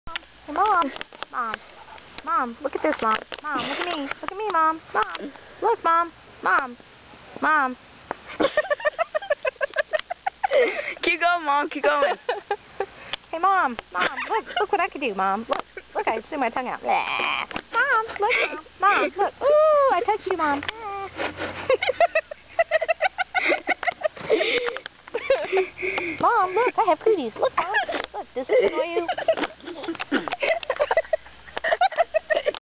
Warm Springs, Georgia Fish Hatchery Turtle Talk
Little son turtle bugs larger mom turtle...a day in the life...